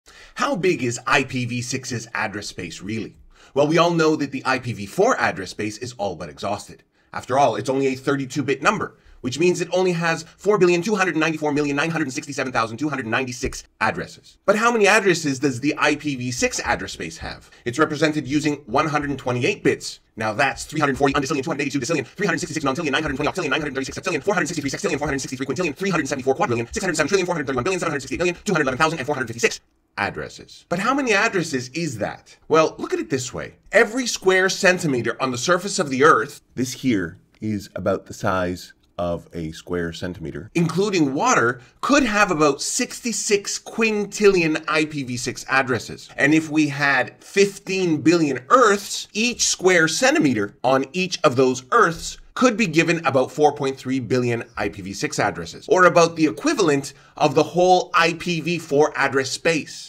How Big is the IPv6 Address Space...really?
IPv6 has an enormous address space, but just how big is it really? 🤯 While IPv4 only has around 4.3 billion addresses, IPv6 boasts 340 undecillion addresses! To put that into perspective, every square centimeter on 15 billion Earths could have an entire IPv4 internet’s worth of addresses. 🌍🌎🌏